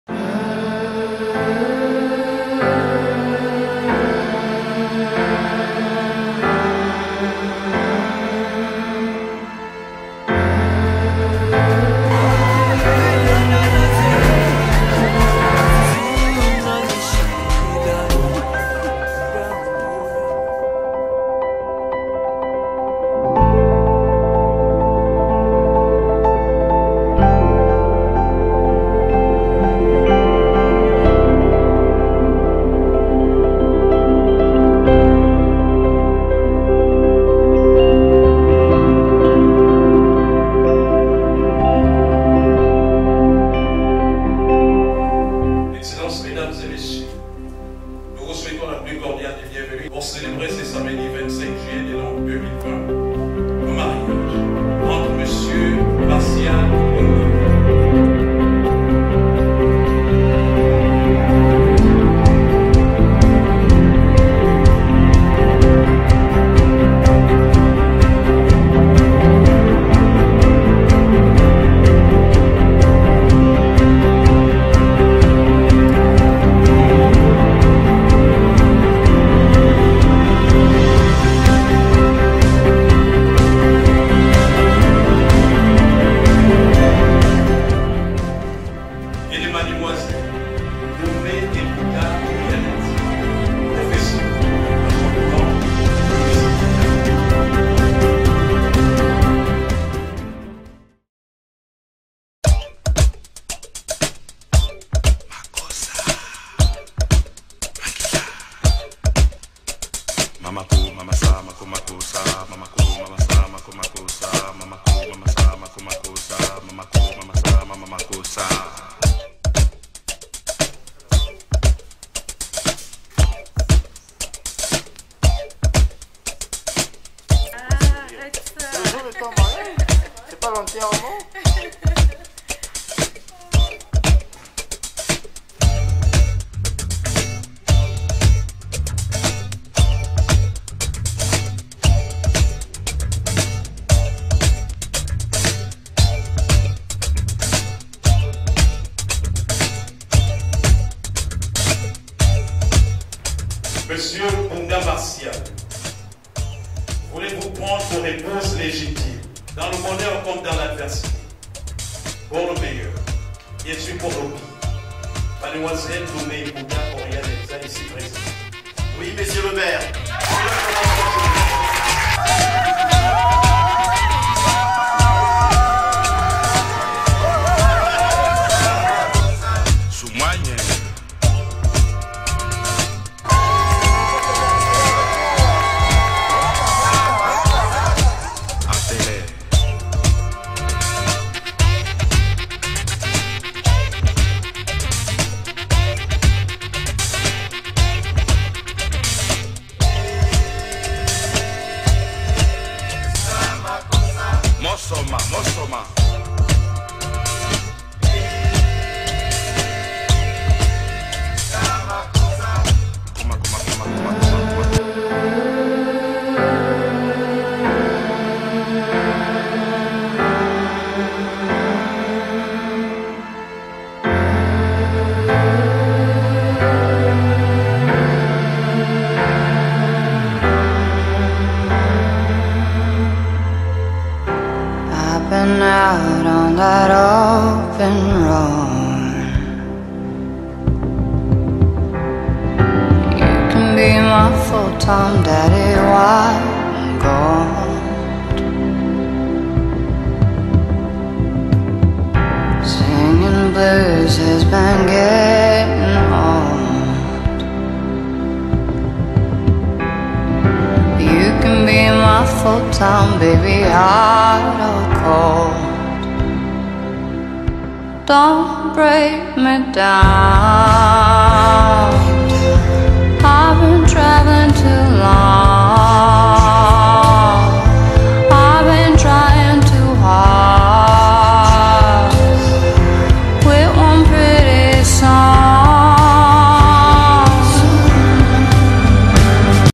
Mariage de Martial & Oriane by Miksoleil
Résumé d'un mariage civil au Gabon entre Martial et Doriane